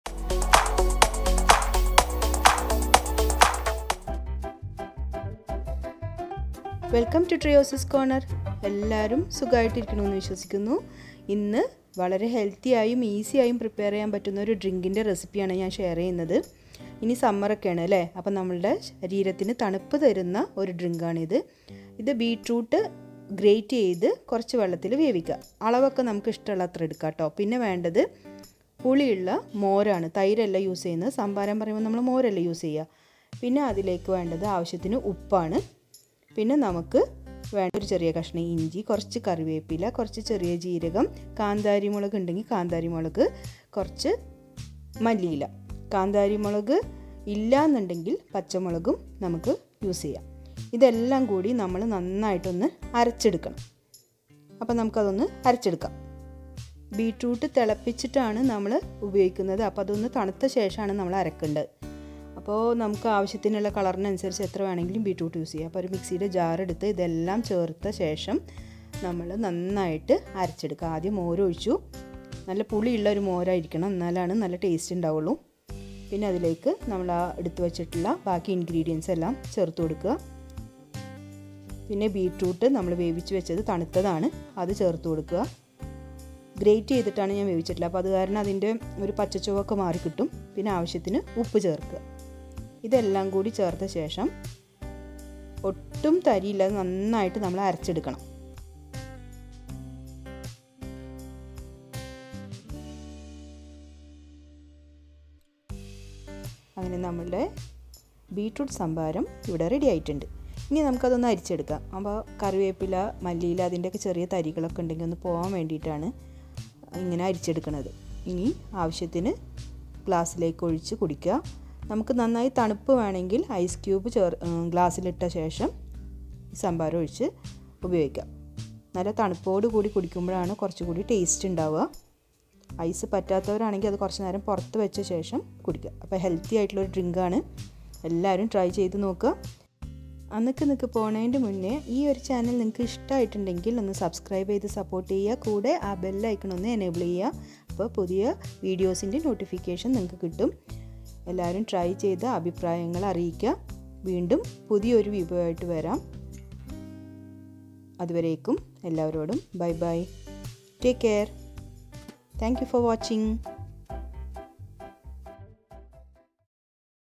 Beetroot buttermilk / Beetroot Sambharam / Healthy drink
A healthy and refreshing Beetroot Buttermilk recipe which makes you cool on a hot summer day.
Beetroot is mixed with buttermilk, ginger, cumin seeds, curry leaves, coriander leaves and green chilies to make a perfect, healthy, tasty buttermilk.